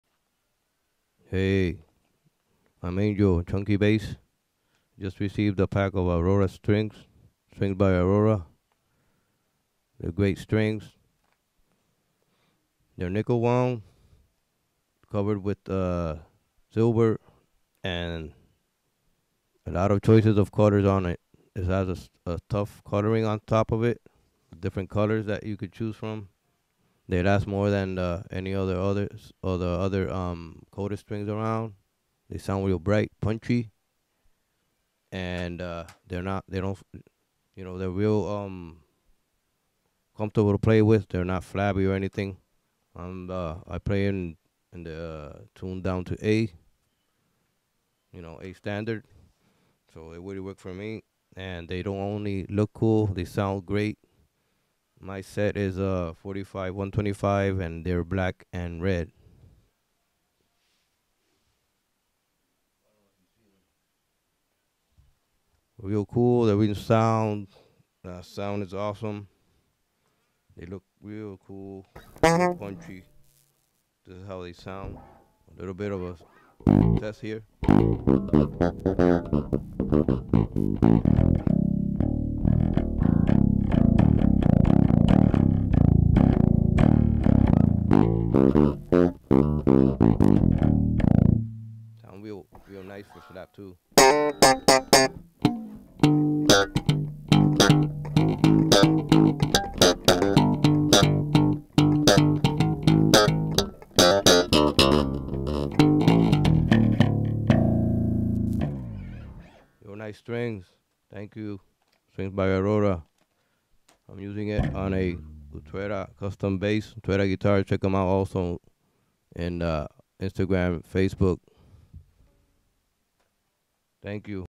Strings by Aurora color coated 5 strings set in Red and Black
Thanks to @StringsbyAurora for the custom set. They are great strings with a great sound and cool colors. I received a red and black colored set for my ​⁠@freddyutrera Classic JMM bass.